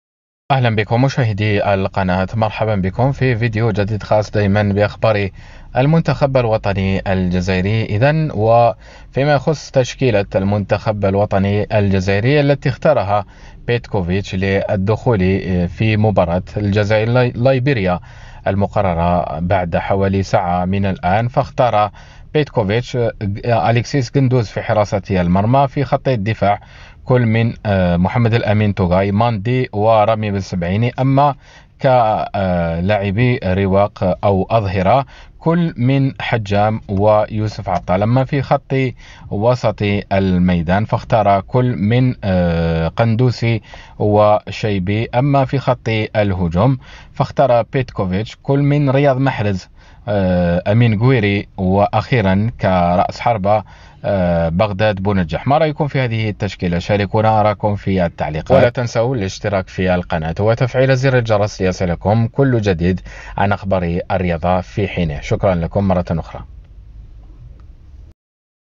رسميا هذه تشكيلة المنتخب الوطني الجزائري اليوم ضد ليبيريا 🇩🇿 على ملعب حسين ايت احمد بتيزي وزو
Equipe national algerie
algerie

تشكيلة المنتخب الجزائري اليوم ضد ليبيريا في مباراة الجزائر ليبيريا